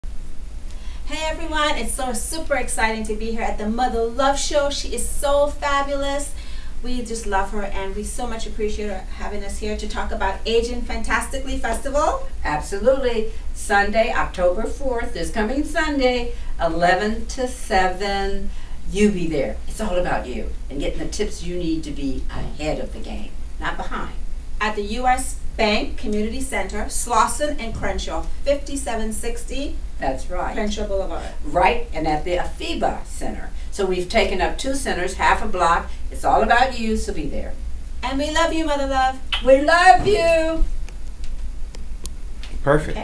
MJ Duffy and Joy Parris
Hi Cyber Babies. Today on LATalkRadio Mother Love talks with MJ Duffy, Founder & CEO of "Aging Fantastically Festival" & PR maven Joy Parris join Mother Love in studio to invite the Los angeles Community to the 1st Annual event to promote health wellness and aging fantastically.
This FREE FAMILY EVENT is Sunday 10/04/15 at the corner of Slauson & Crenashaw.
About Joy
richgirlproductions.co